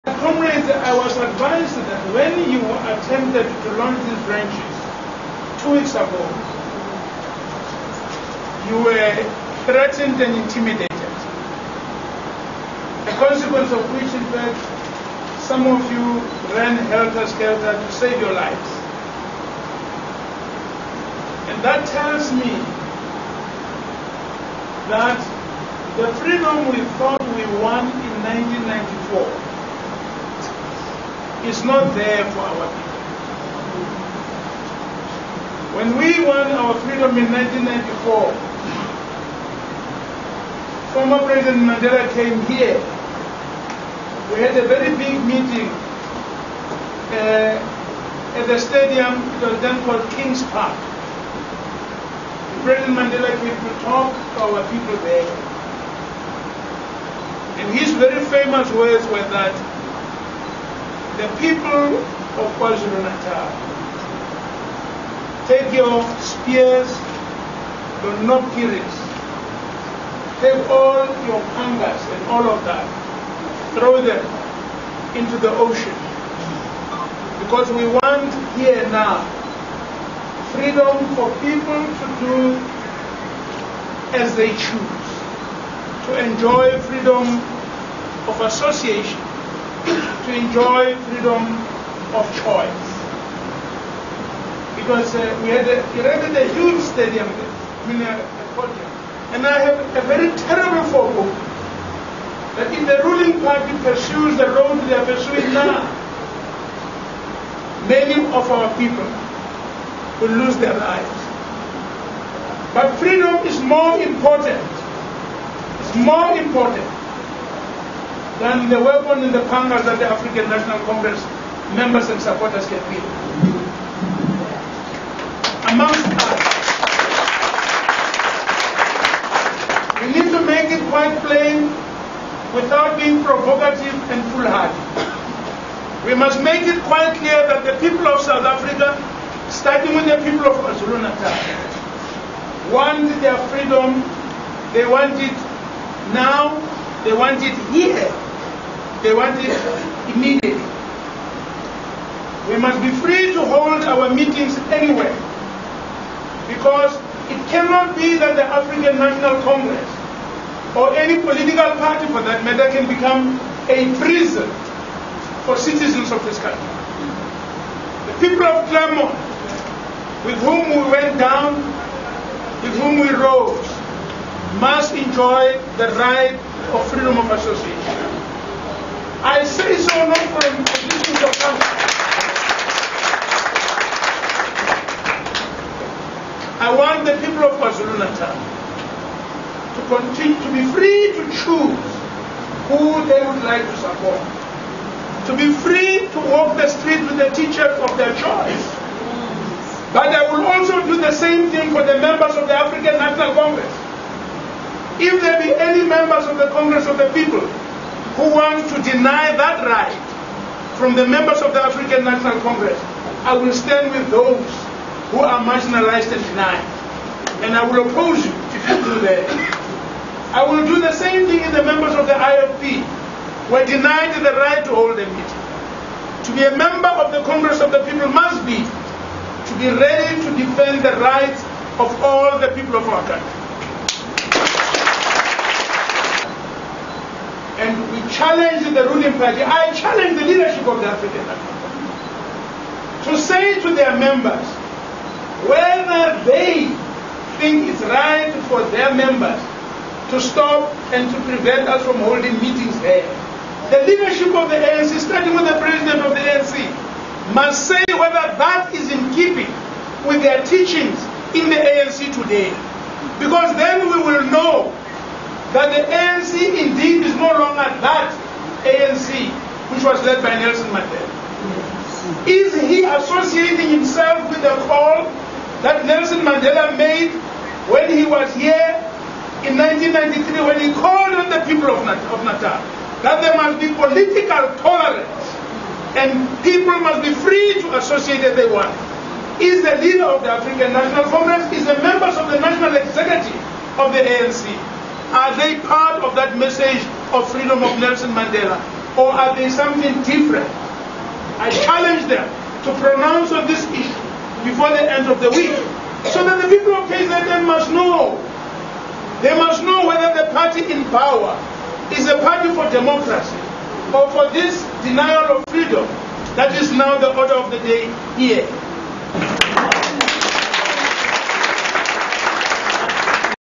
TERROR TALKS ABOUT FREEDOM OF CHOICE.mp4
president lekota speaks at cleremont launch